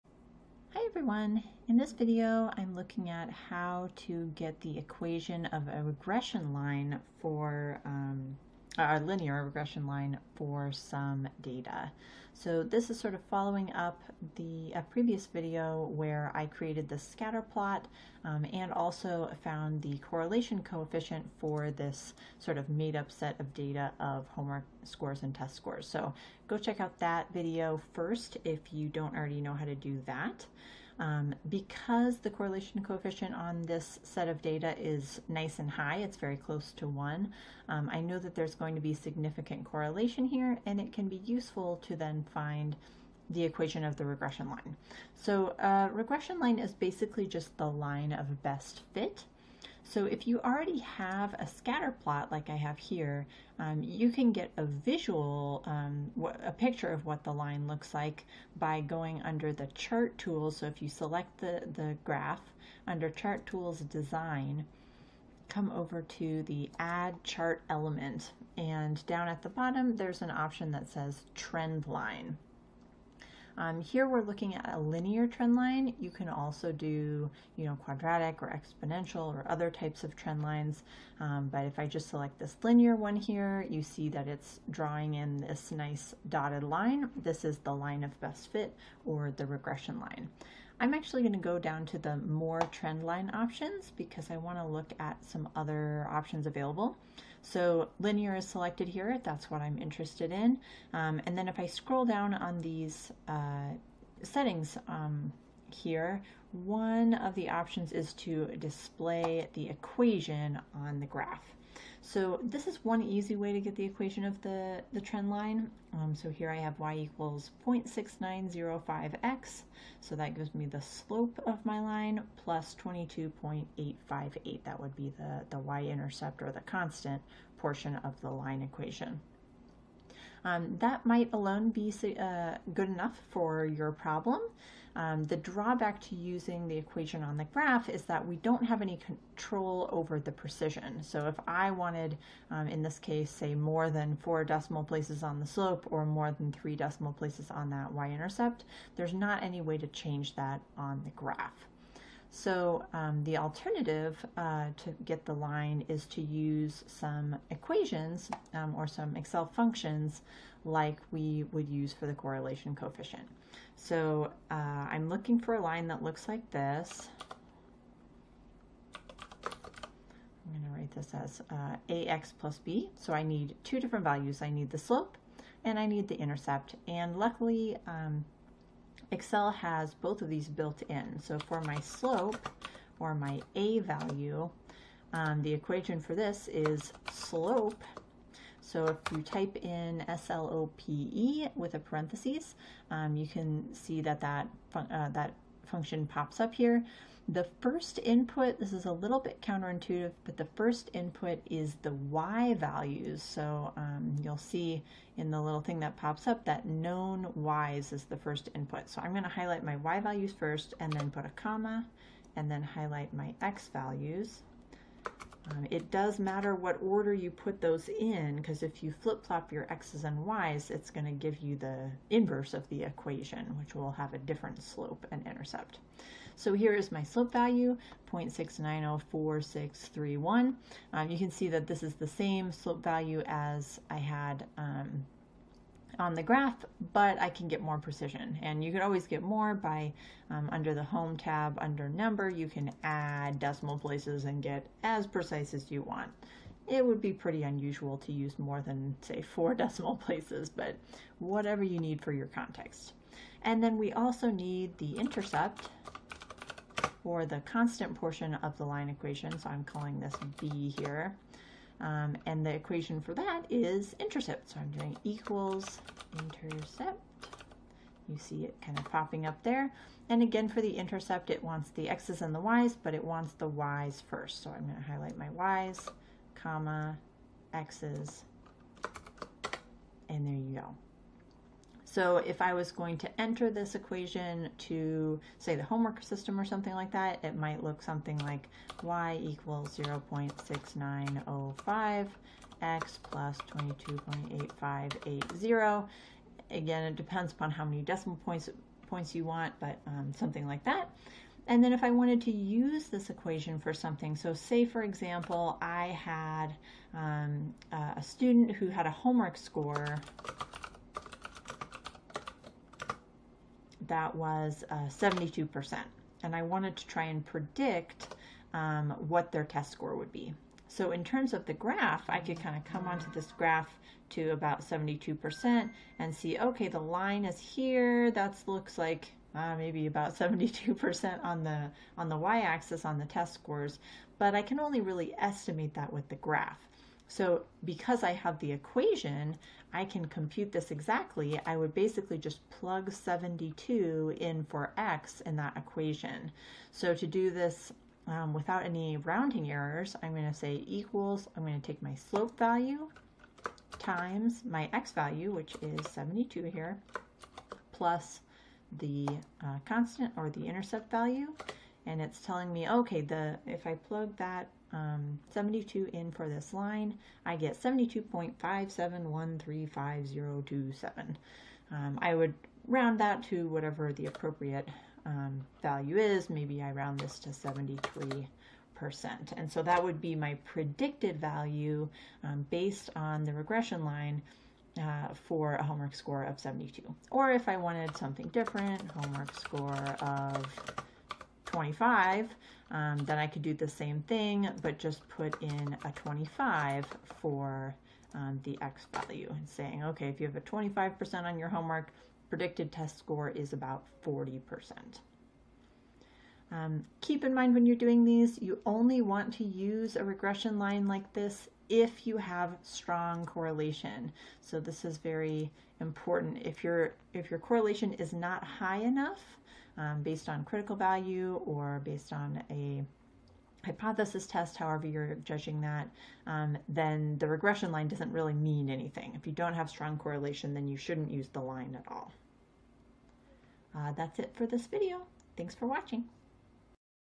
Regression Lines - Excel 2019
Get the equation of the least squares regression line (line of best fit) for your data set using Excel.